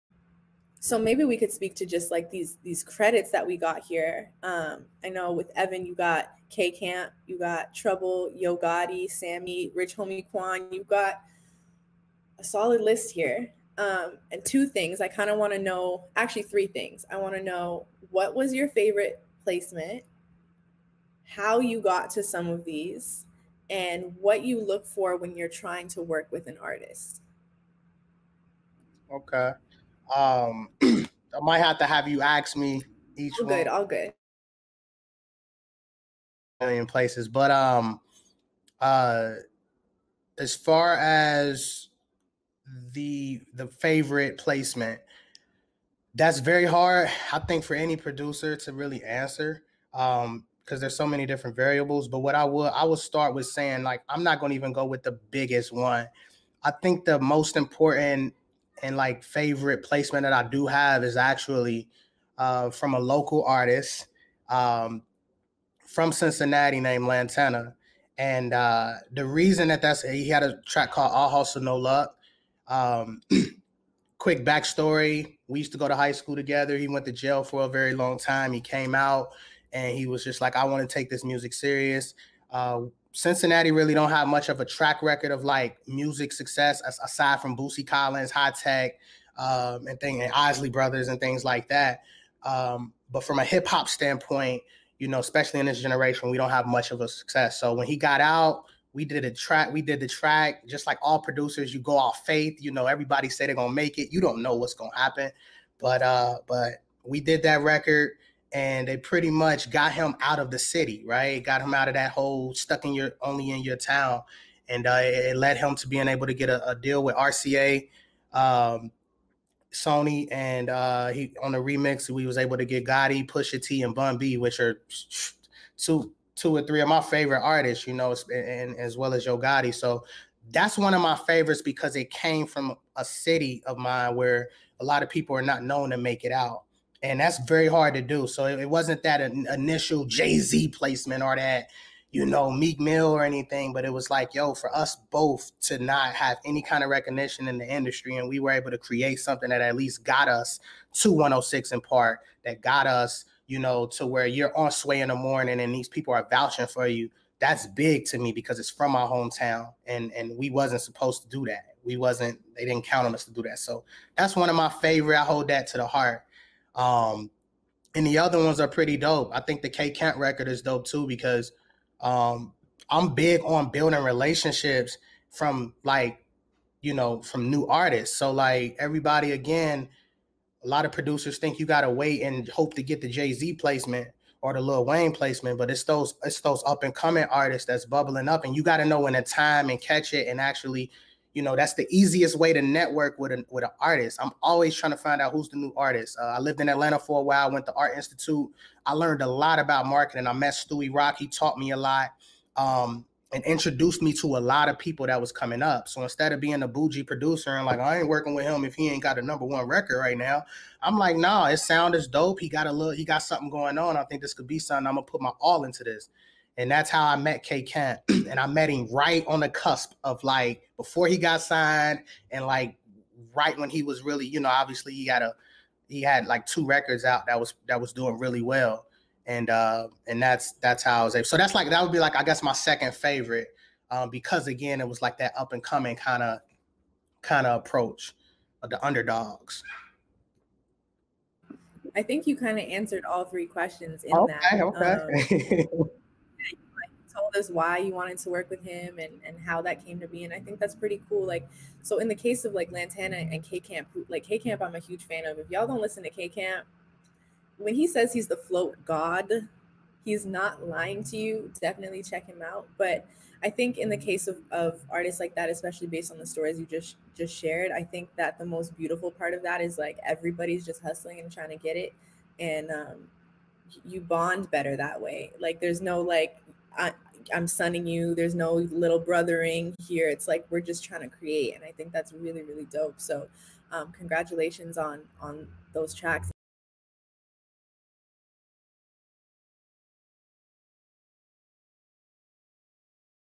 The Importance of an Artist Producer Relationship: MamaNamedMe Evan
Learning more about the importance of networking and the artist producer relationship.

Don't forget to tune in for more stories from our BeatStars Publishing clients on The Deal, every Tuesday at 3PM EST!